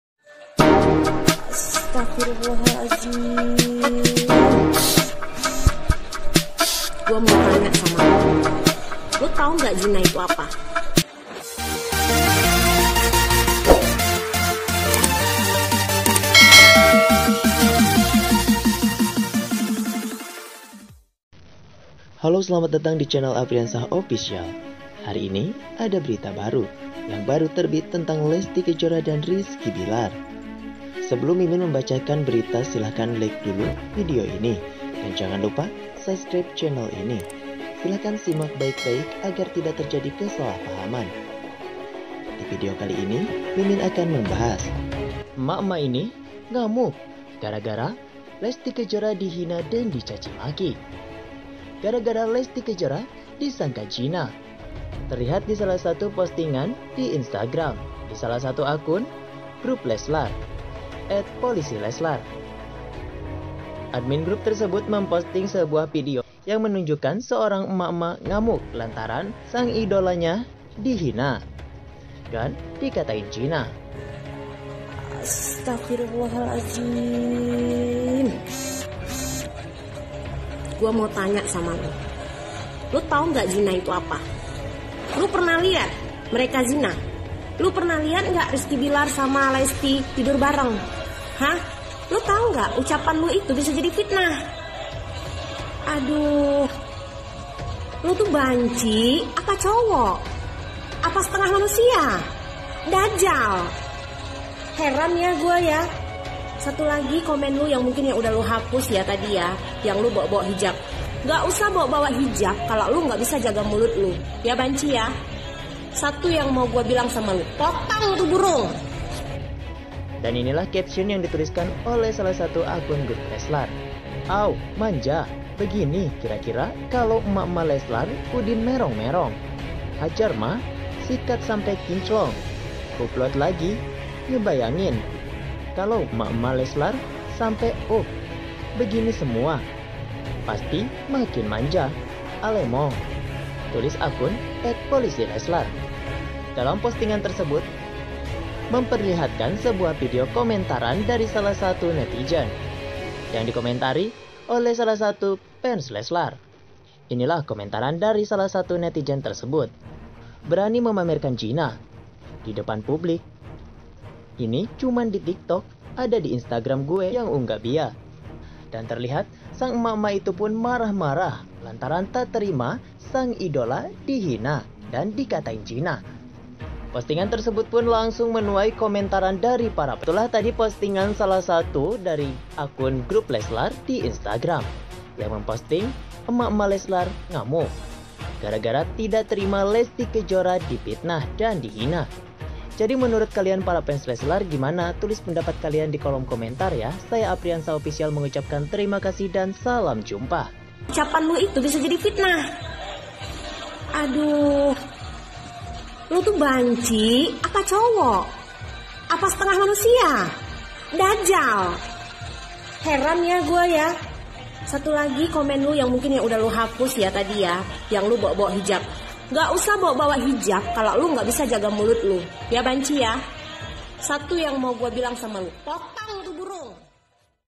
Hebooohh...Emak Emak Ini Ngamuk Tak Terima Lesti Kejora Di Fitnah..
terlihat dari salah satu postingan di Instagram di salah satu grup fans Leslar.

Ada seorang emak-emak yang ngamuk lantaran tidak terima sang idola difitnah